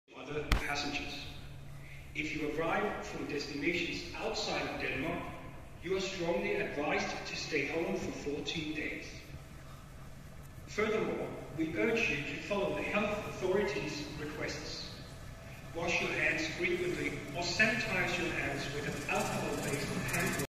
Coronavirus, l'aeroporto di Copenaghen è spettrale
Immagini girate il 22 aprile 2020.